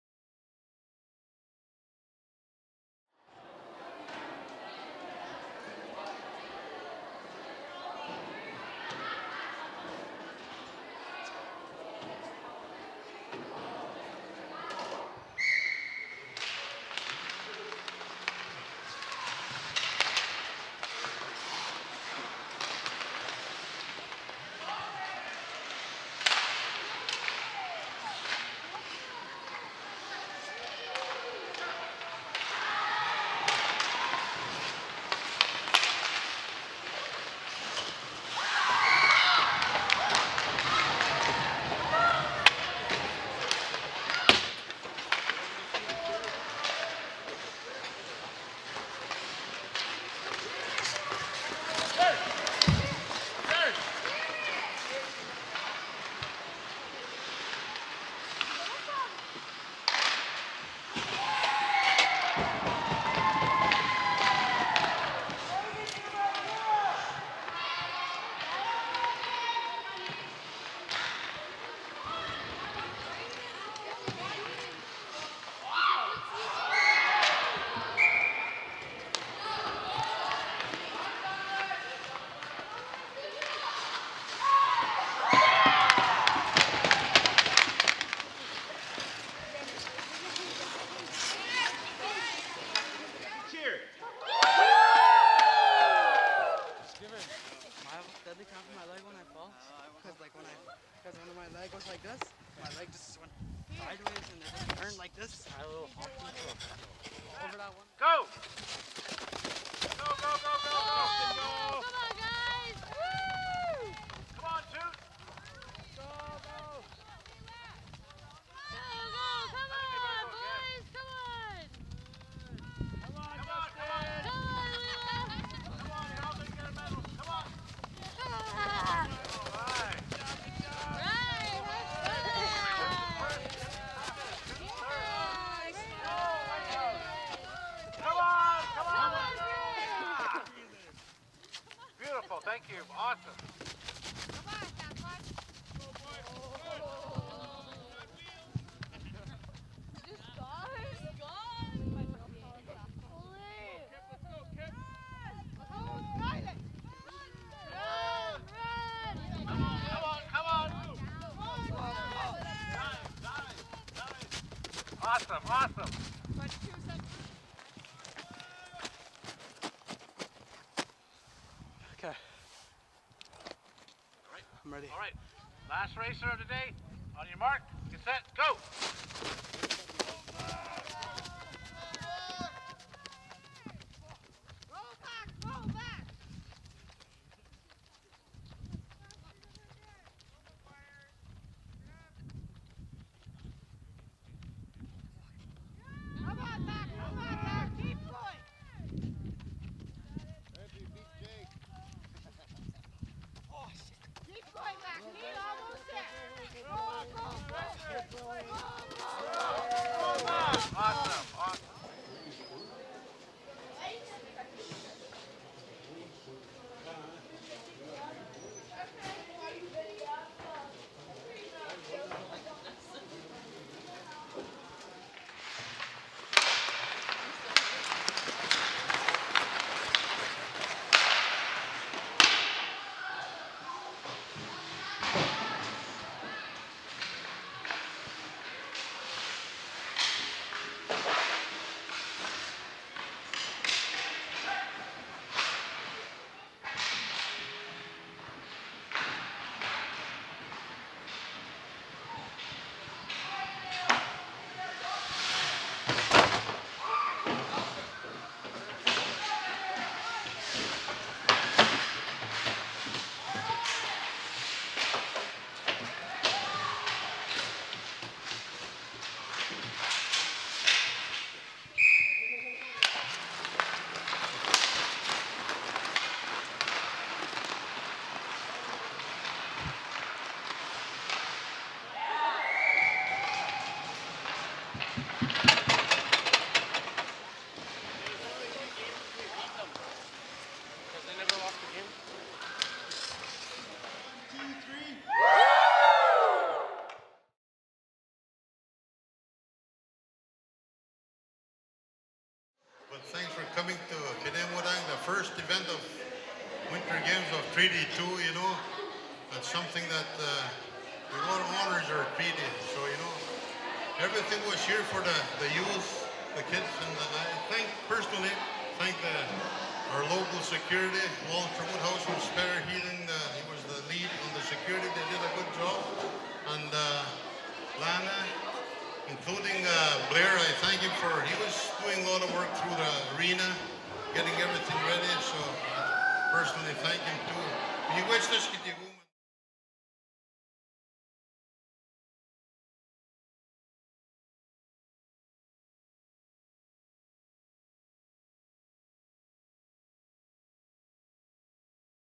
2020 Winter Games Highlights and Closing Ceremonies
Treaty 2 Territory - The Government of First Nations in Treaty 2 Territory held the 2020 Winter Games on February 26-29, 2020 in Pinaymootang First Nation. The 2020 Winter Games is the first of it's kind within the numbered Treaties. 330 youth from the Treaty 2 Territory gathered in a celebration of unity through sports. The FNT2T is educating youth of the importance of the Treaty signed between the Anishinaabe and Crown in 1871. Uniting our youth is an important step in moving forward as a Nation. This video is a highlight of the 2020 Winter Games. 2021 will mark the 150th Anniversary of the signing of Treaty 2. We will be announcing the 2021 Summer Games and Celebration shorty. The FNT2T would like to say Miigwetch to everyone involved with Winter Games!!! or on Facebook at Anishinaabe Agowidiiwinan.